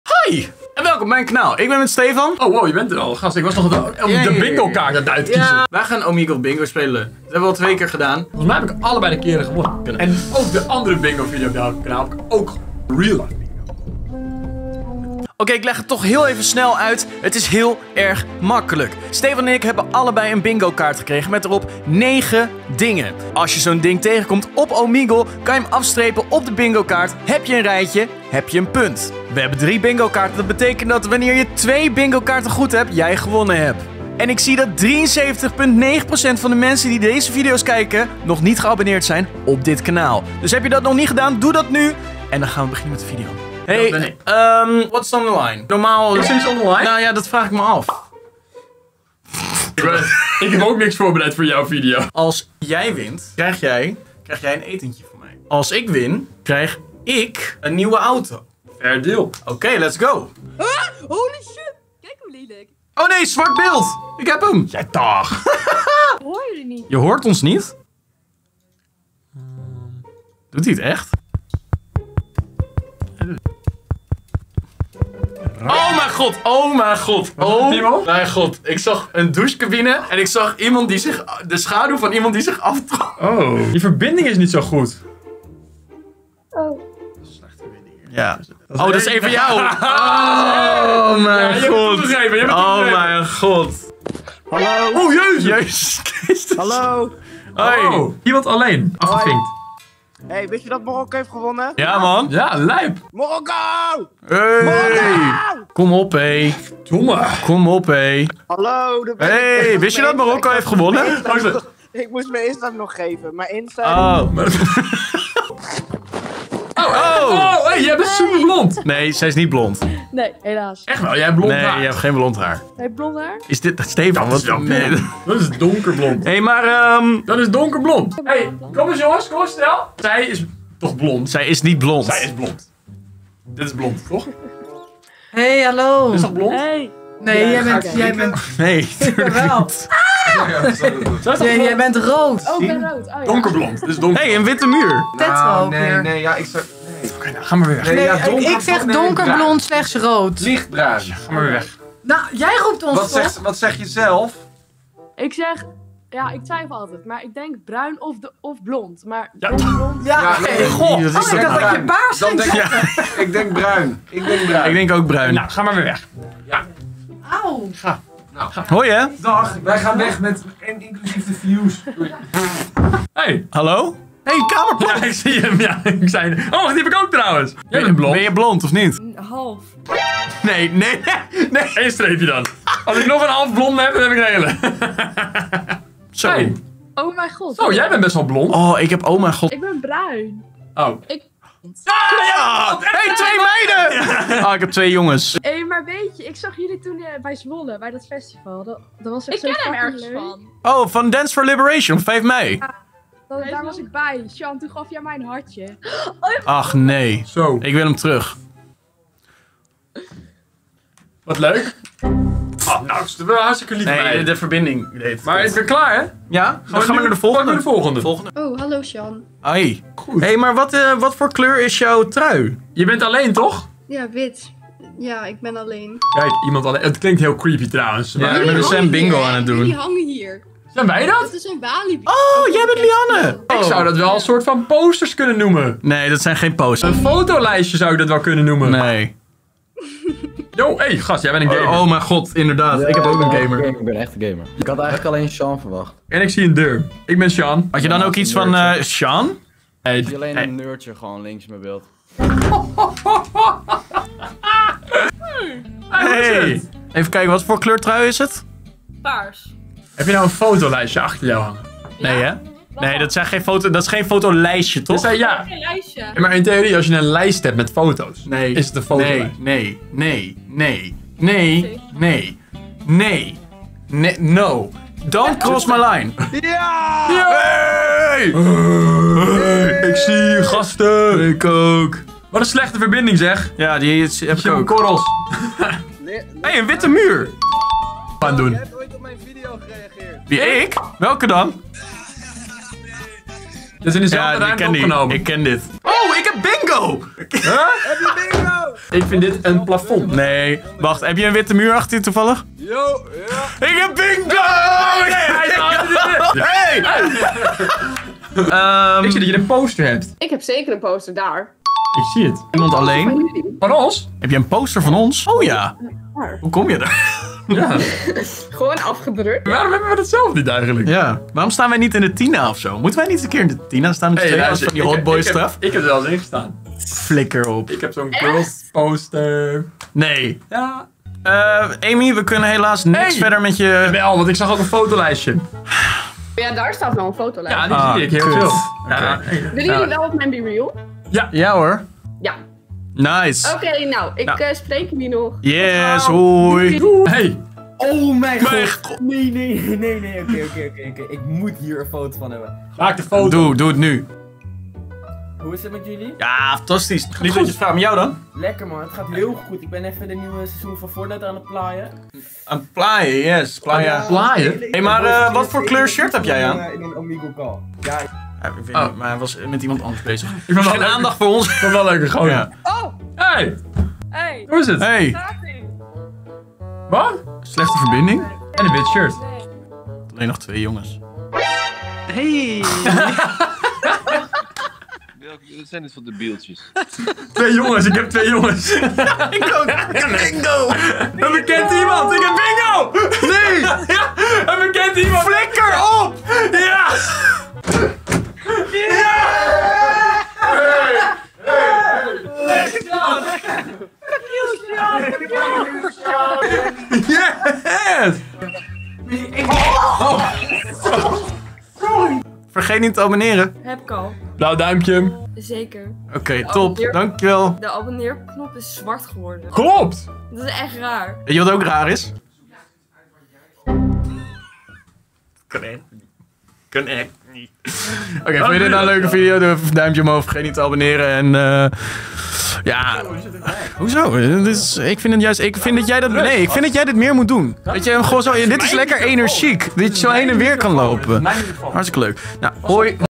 Wie Komen Wij Tegen?! - Omegle Bingo
Deze week speelde ik Omegle Bingo